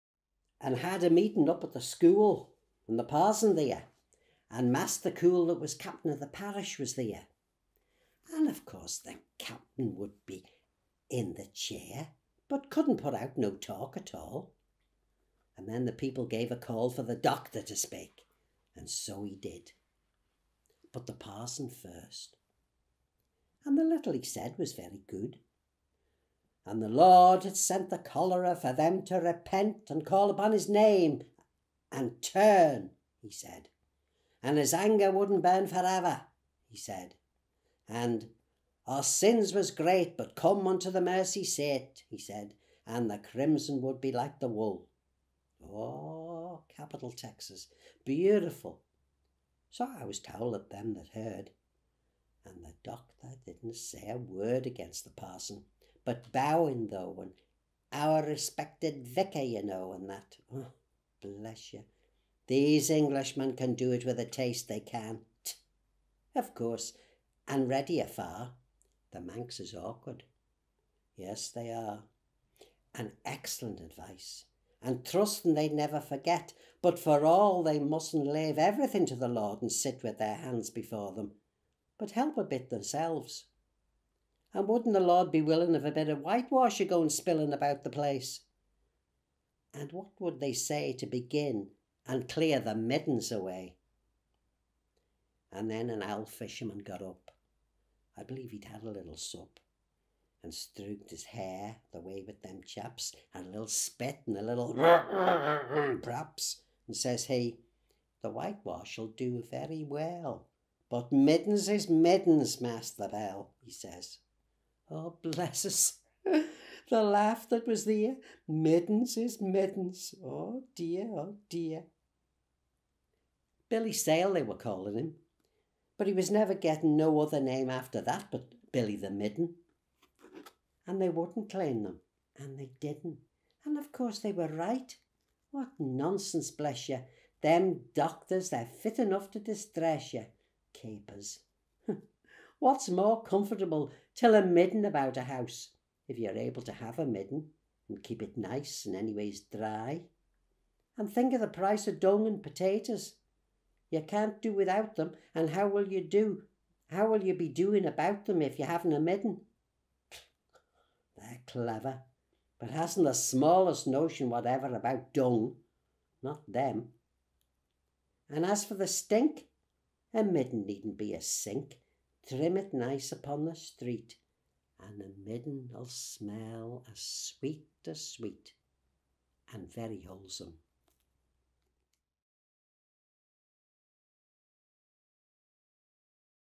"Middens is middens, Master Bell!" - An extract from T. E. Brown's Manx dialect poetry
An extract from one of the greatest poems by the Manx National Poet, T. E. Brown.
Considered by some Brown's masterpiece, 'The Doctor' is part of Brown's brilliant Fo'c's'le Yarns series of Manx dialect poems. This series of poems is considered the cornerstone of Manx literature, and the highest point of Manx dialect poetry.

This extract from the long narrative poem is read by John Cashen OBE.
It was originally recorded for at the conclusion of an oral history recording made with John, for former Chief Financial Officer of the Isle of Man government.

A full recording of this same poem was made by Phil Gawne in 2020 and is available